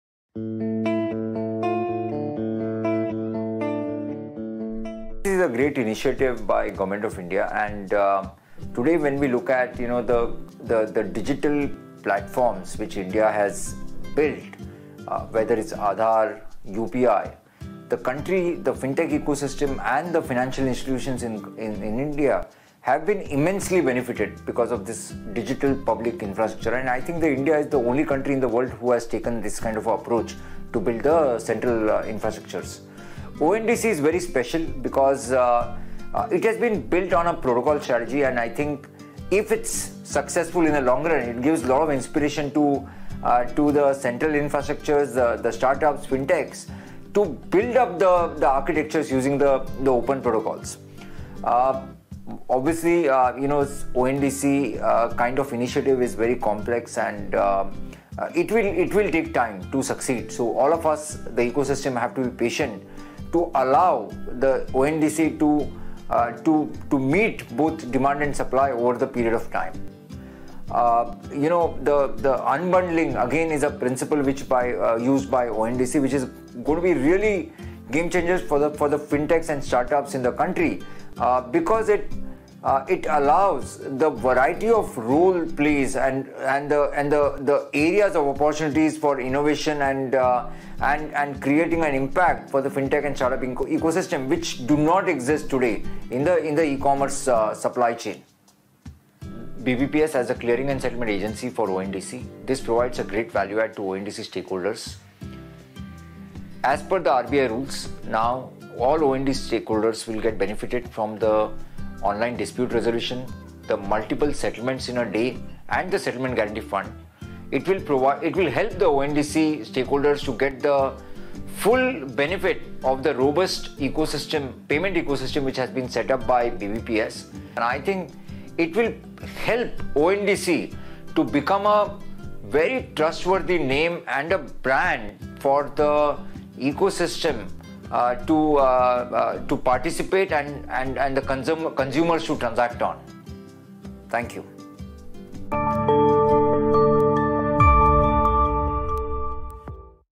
Foundation Day | Dilip Asbe, MD & CEO, National Payments Corporation Of India
Dilip Asbe, MD & CEO, National Payments Corporation Of India (NPCI) , also an esteemed member of ONDC’s Board & Advisory Council shares his vision since the inception of ONDC and the new wave of opportunities this initiative will unlock for enabling democratized and inclusive e-commerce over the coming years.